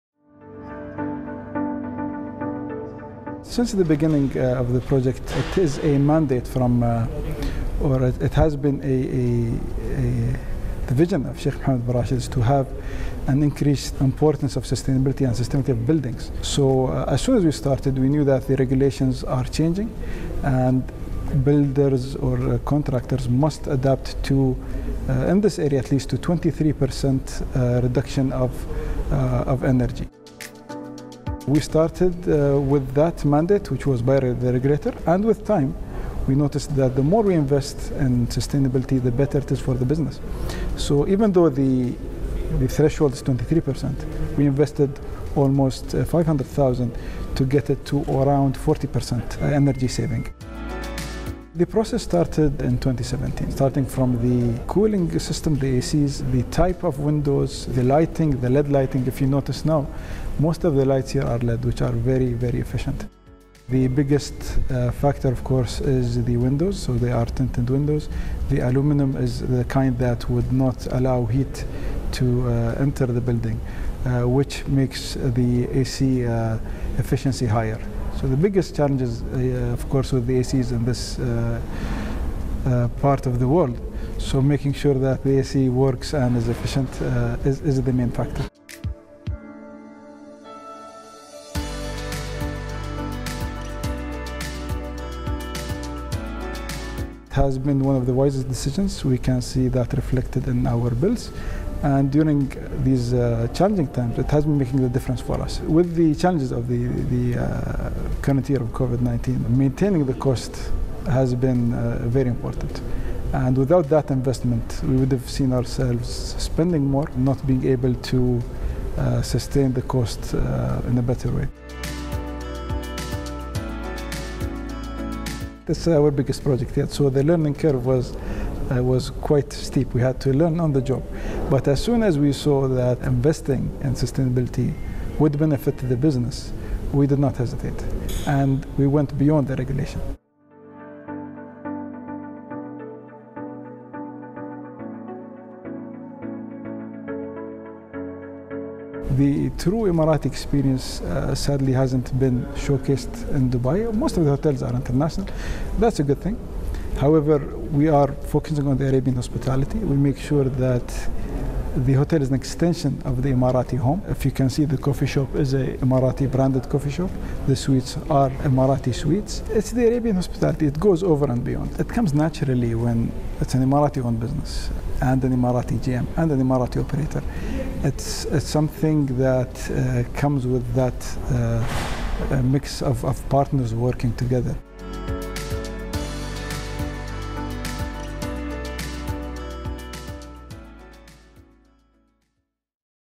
First hotel entirely run by Emiratis in Dubai provides feel of an Emirati home
Mohammed Hanif, founder of The Manor by JA, says the hotel is run entirely by Emiratis and offers local hospitality with an eco-friendly twist